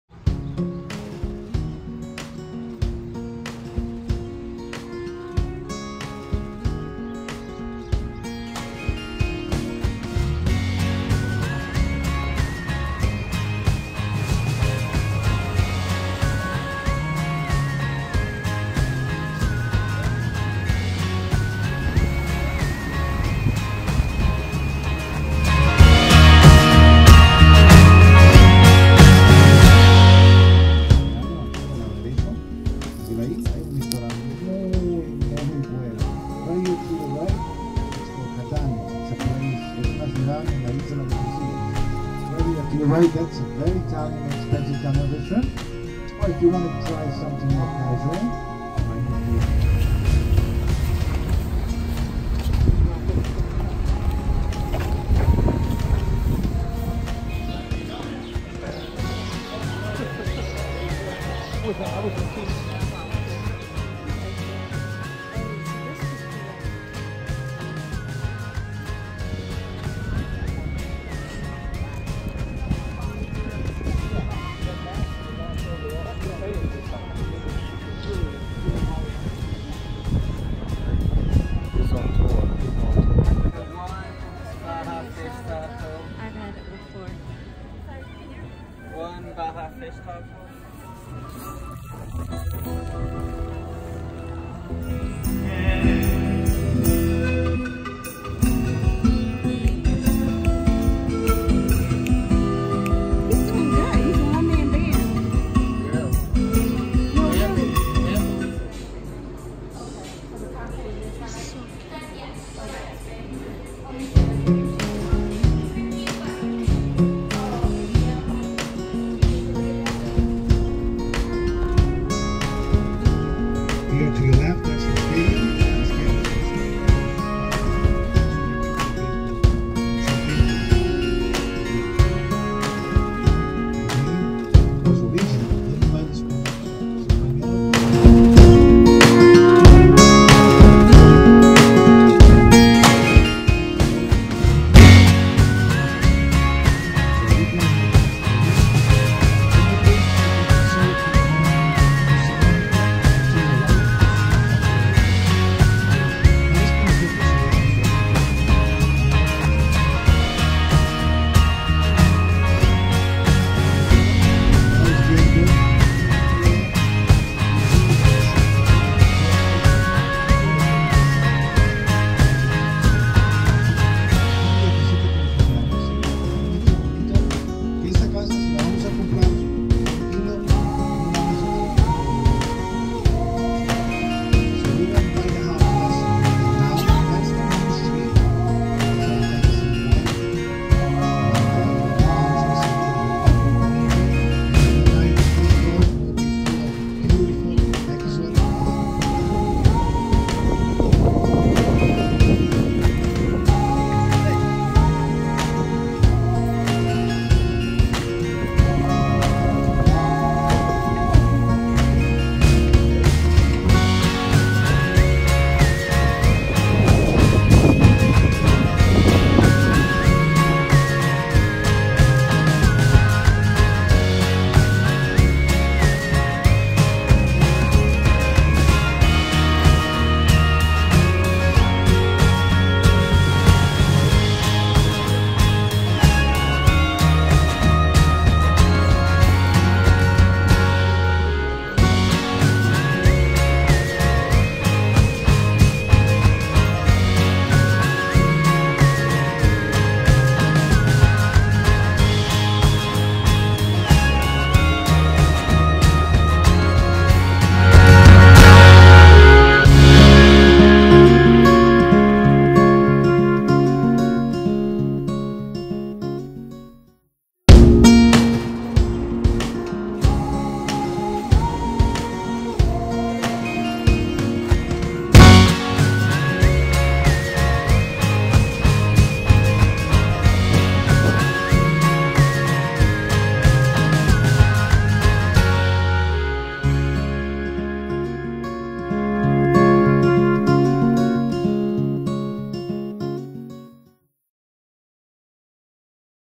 San Diego | May 2019 | Day 4
Hello travelers! In this video, we take you with us to San Diego via a small tour bus (about eight sightseers of us in total). We see beautiful places like Coronado, La Jolla, and the U.S.S. Midway as well as eat at authentic restaurants with live entertainment and cupcakes to end the sweet day!

Happy Travels!